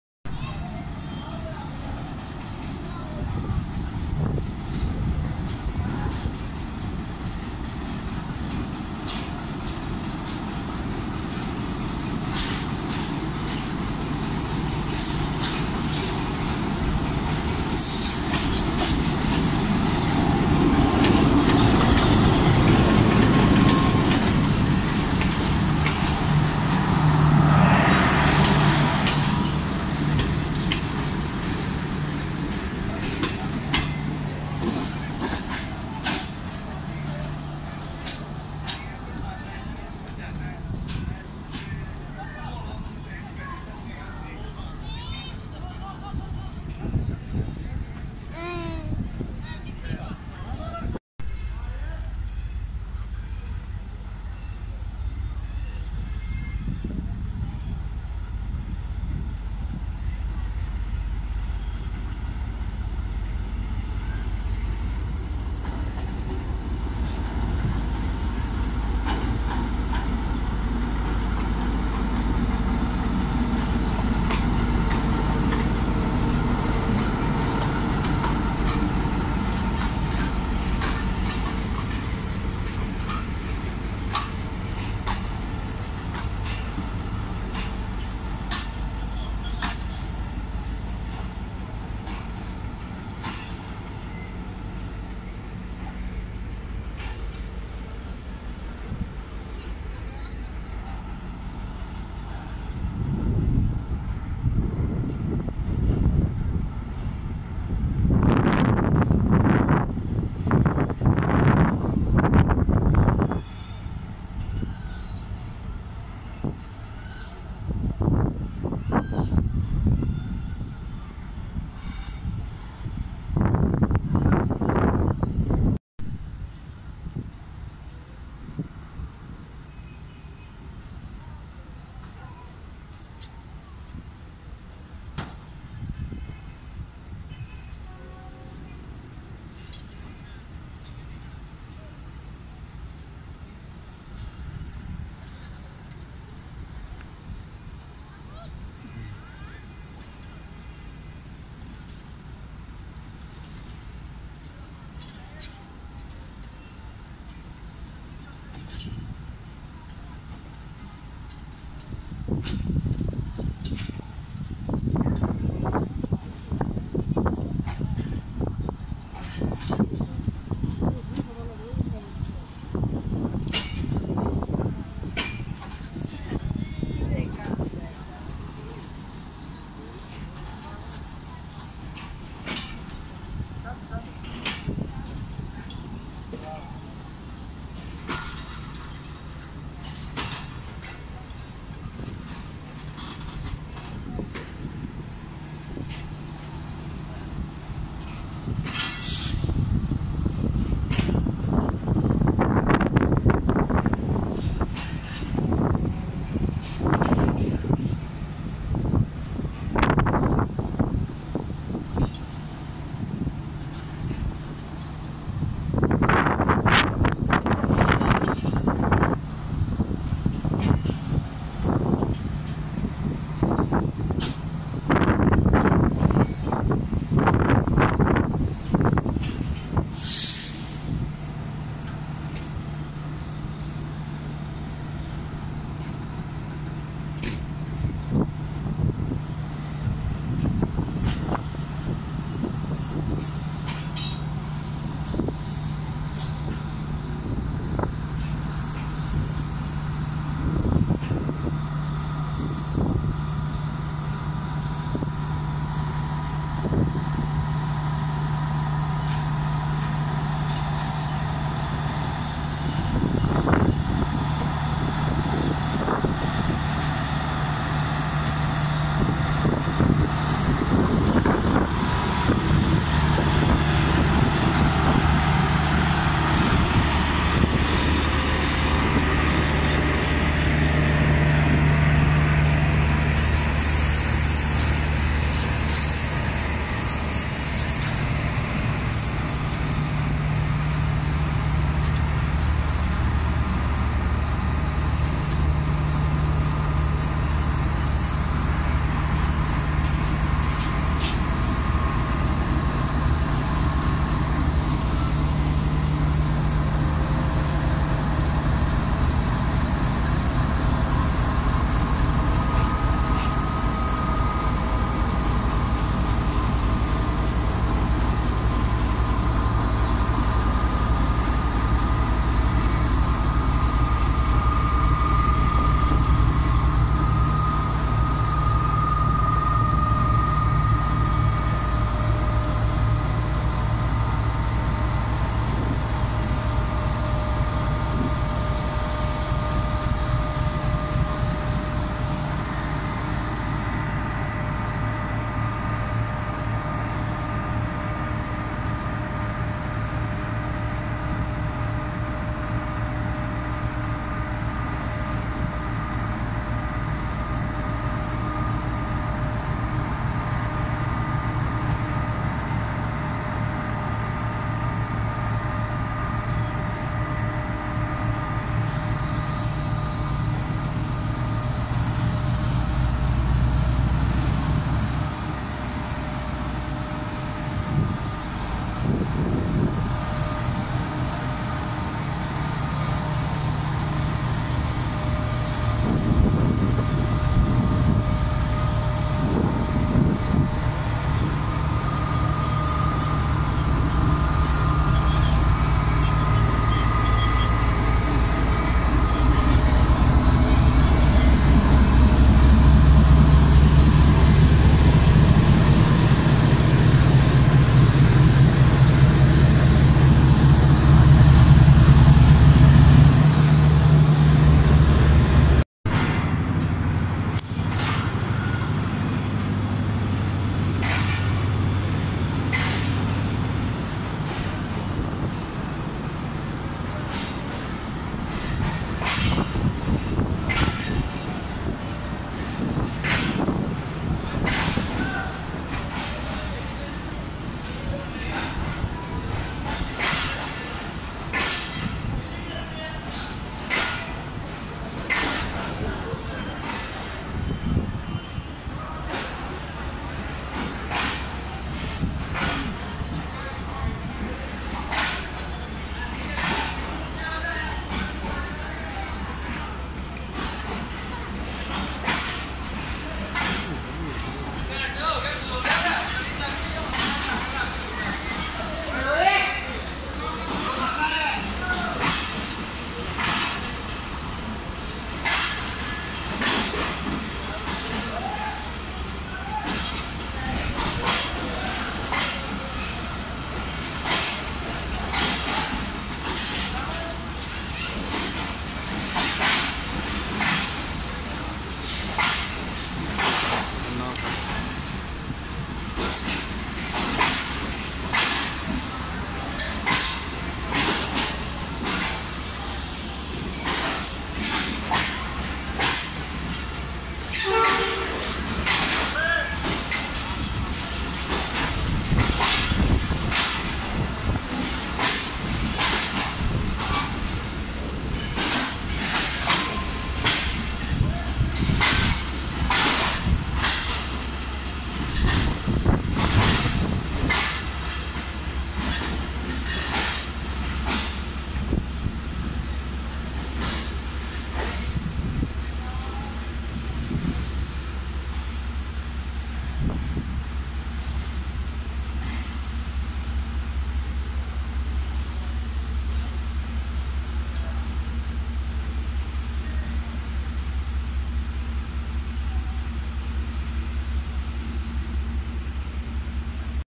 Shunting process at Faisalabad Railway Station of Rehman baba coaches || Pakistan Railways
Hi Everyone welcome to my another video Railfanning video Rehman baba Express added coaches at Faisalabad Railway Station shunting process attached new power geu-40 😍